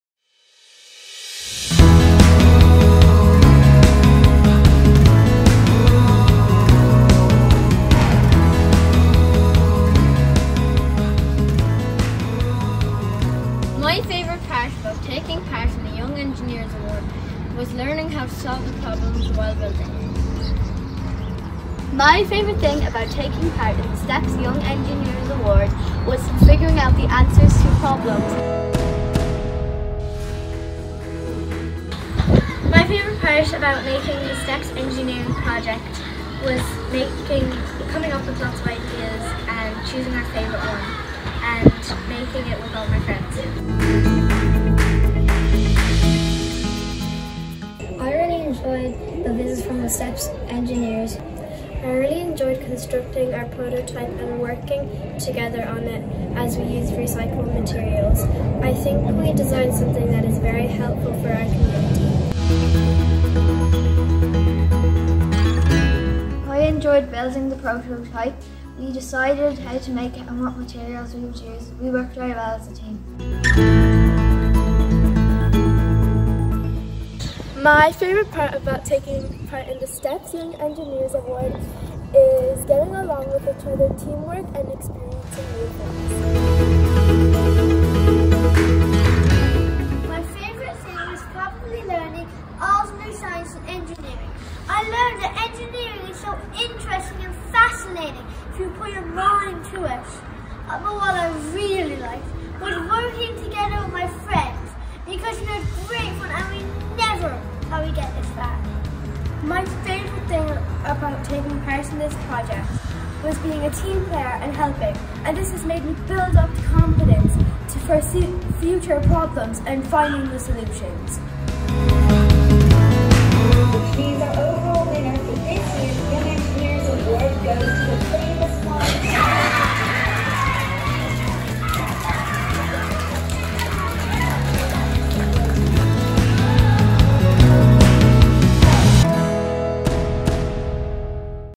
Register for STEPS Young Engineers Award 2024/2025!
Hear from our previous STEPS Young Engineers Award finalists and find out why you should register your class for the STEPS Young Engineers Award 2024/2025. Join in the fun this coming September!